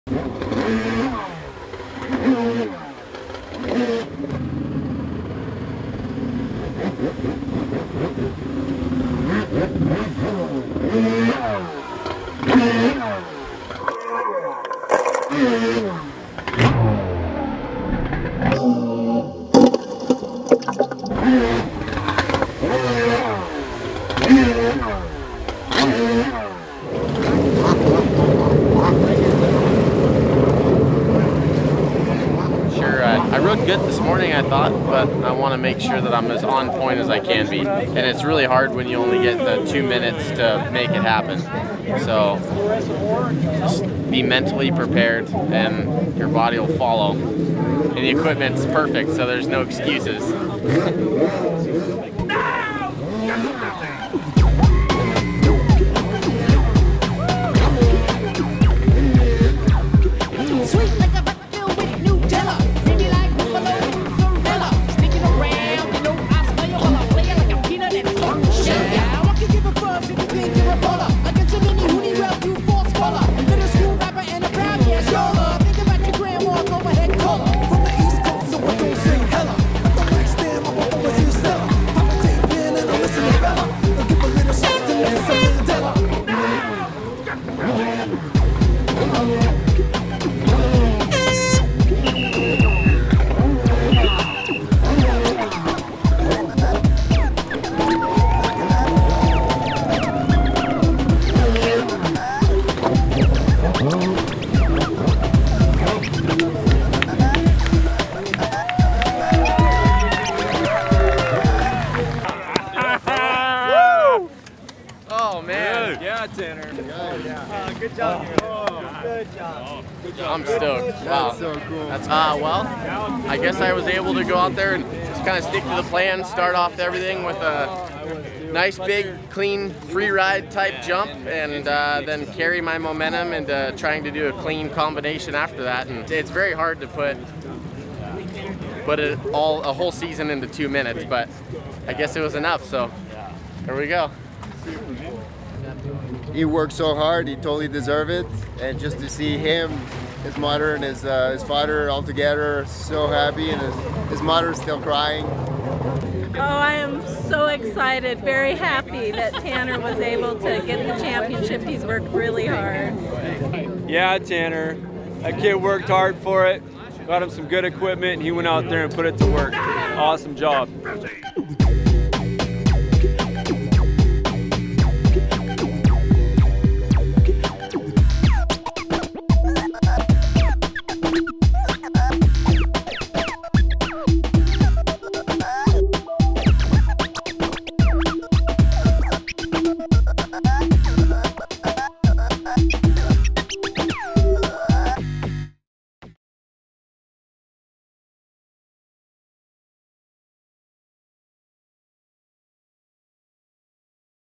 Tanner Thomas 2015 IJSBA Amateur World Champion Rickter XFS Ninja Competition
Congratulations to Rickter-RRP Rider Tanner Thomas for his World Championship on IJSBA Amateur class on Havasu Lake.